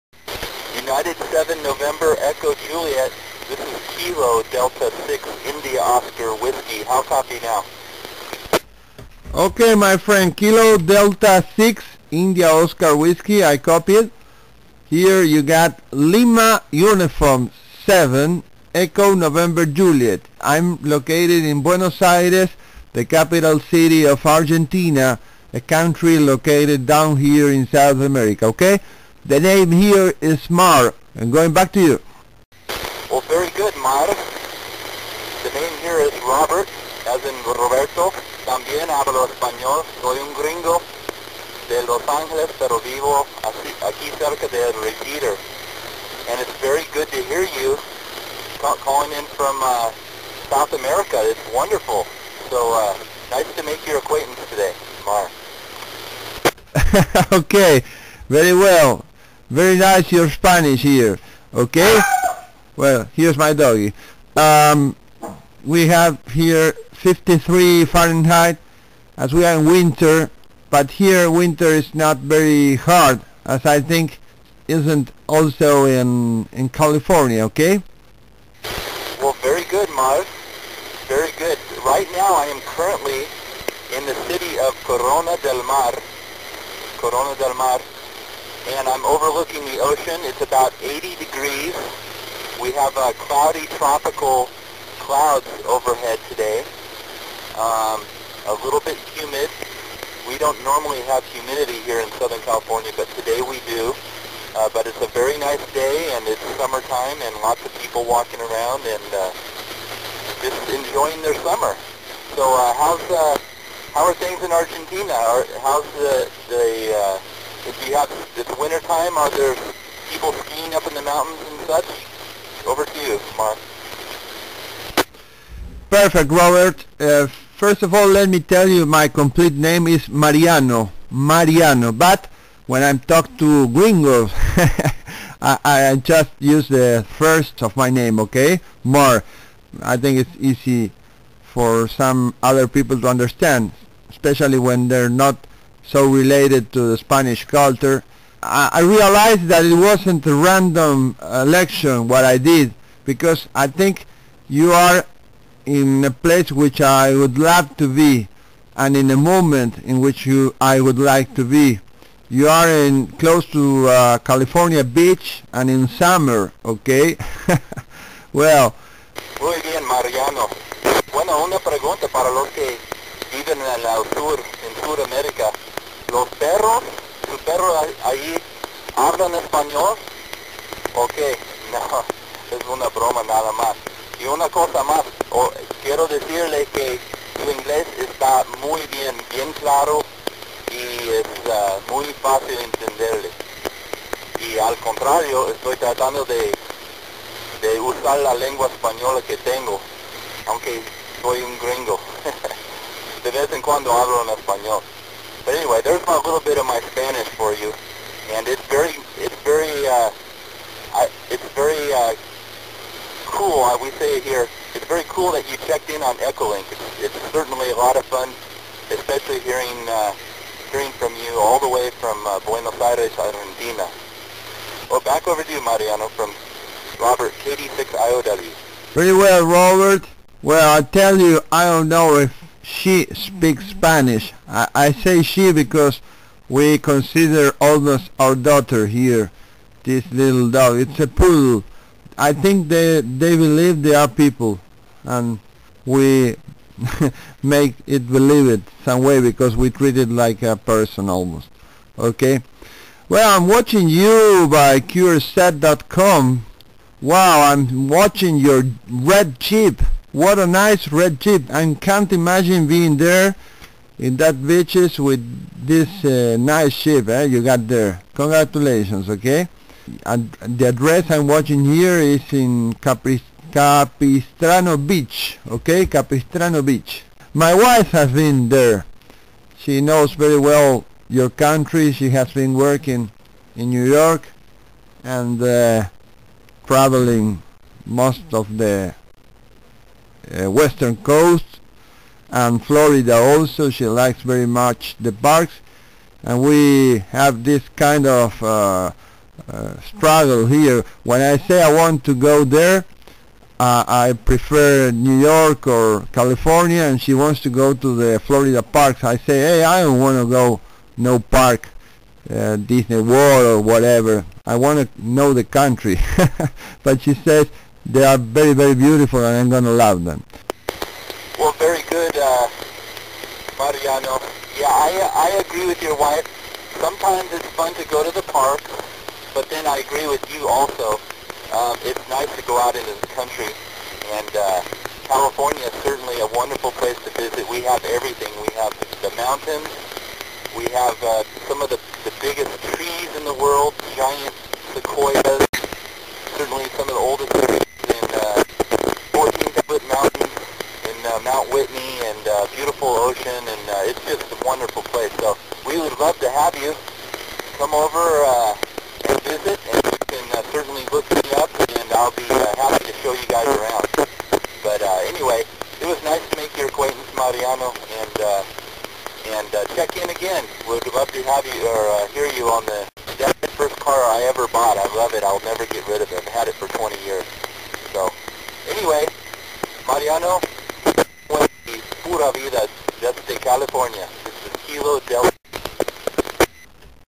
QSO KD6IOW -12 de Julio 2012
QSO via K6SOA-r (repetidora en San Clemente- California USA)
con la estación KD6IOW de Robert en Corona Grande - CA. U.S.A.